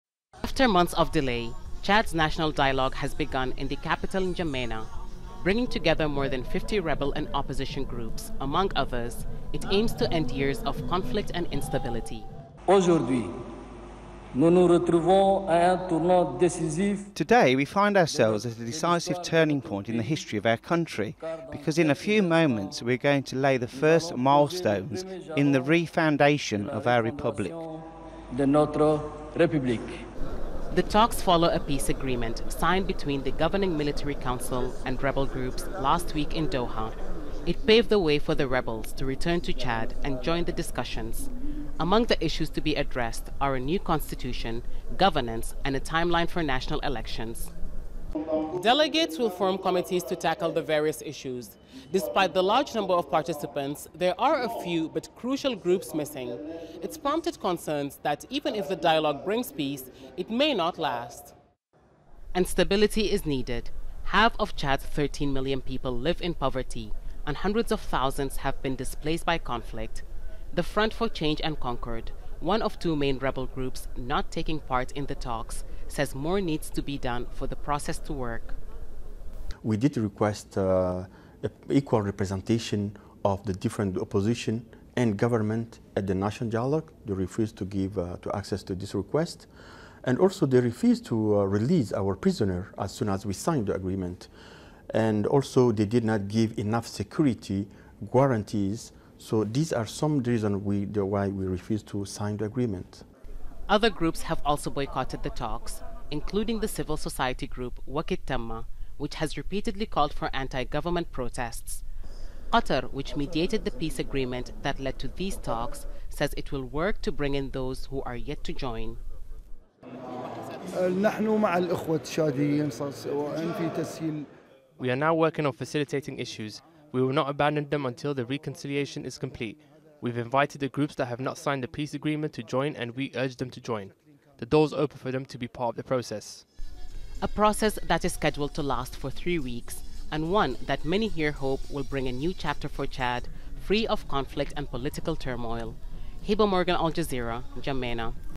Chad starts ‘national dialogue’ to pave way for elections
Chad's military leadership has started talks, with rebels, opposition parties, and civil society groups.
It is hoped the negotiations will end the violence and pave the way for elections.
But some groups are refusing to take part.
Al Jazeera's @Hiba Morgan reports.